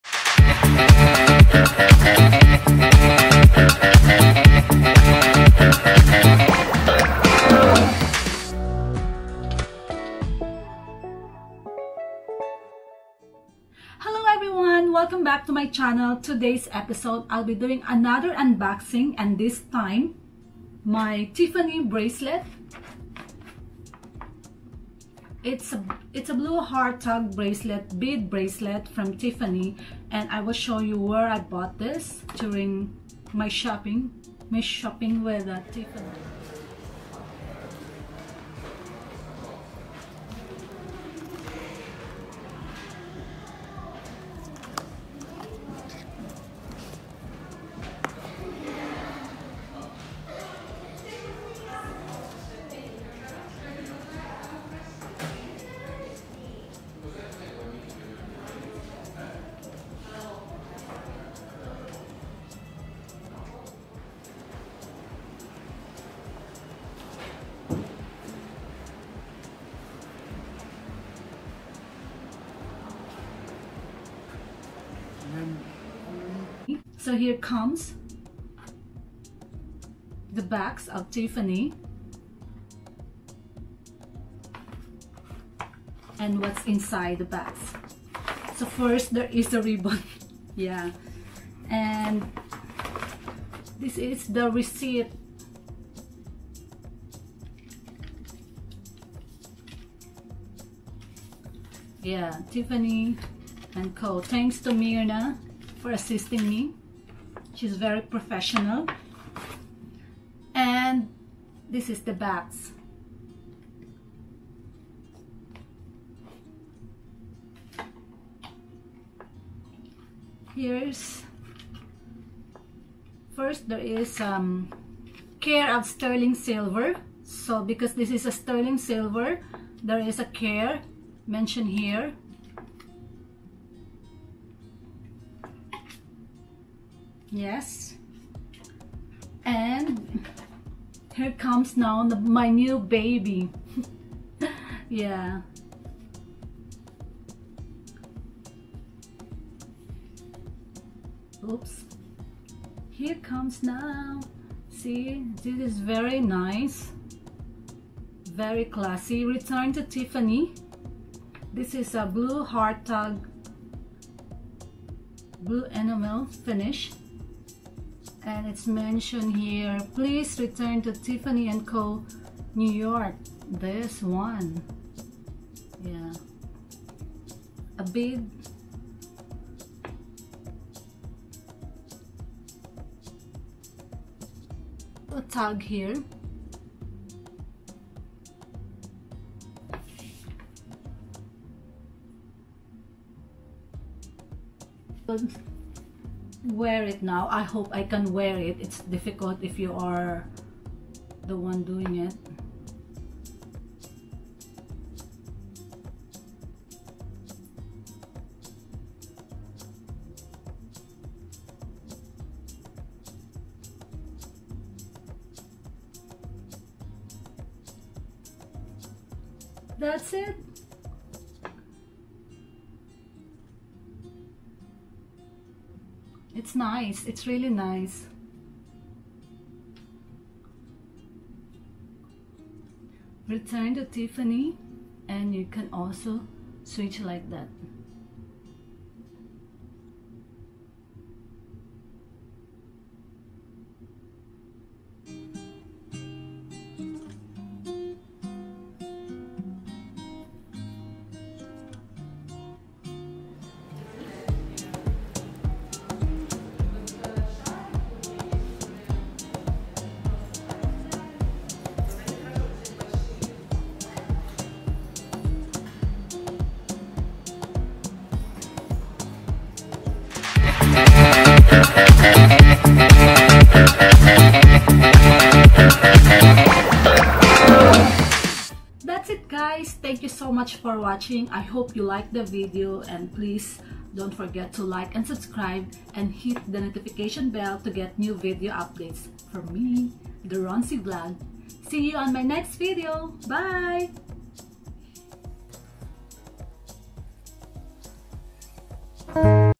Return to Tiffany Blue Heart Tag Bead Bracelet Unboxing I Tiffany & Co.
Unboxing Return to Tiffany Heart Tag Bead  Bracelet

Tiffany Blue™ Heart Tag Bead Bracelet
in Silver, 4 mm

Description & Details
Inspired by the iconic key ring first introduced in 1969, the Return to Tiffany collection is a classic reinvented. An elegant bead bracelet and engraved tag combine to create a simple and timeless design.
Sterling silver tag with Tiffany Blue enamel finish
On a bead bracelet
Size medium
Fits wrists up to 6.25"
Beads, 4 mm
Instantly recognizable, the signature Tiffany Blue hue of this design’s enamel finish has been as iconic as the brand itself since its founding in 1837

For my Return to Tiffany Double Heart Tag necklace with a Diamond Mini, please watch this

RHONZIE VLOG
All about travel, lifestyle and fashion.